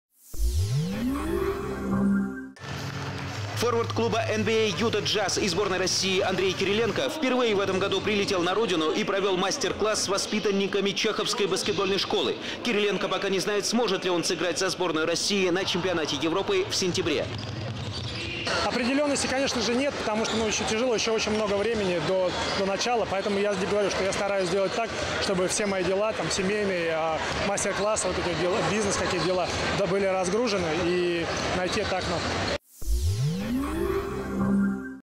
Баскетбол. Кириленко в Москве!
Форвард Utah Андрей Кириленко впервые за год приехал в Москву, чтобы провести мастер-класс.